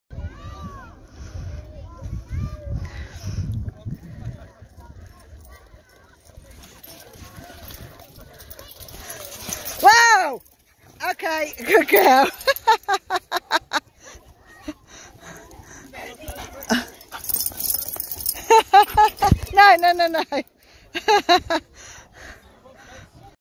That's what you call good recall with a Czechoslovakian Vlcak Wolfdog called Tala!
Tala the Czechoslovakian Vlcak Wolfdog runs back across the beach from human dad and sister ignoring everyone else to get back to her dog sister Skye and human mum - me!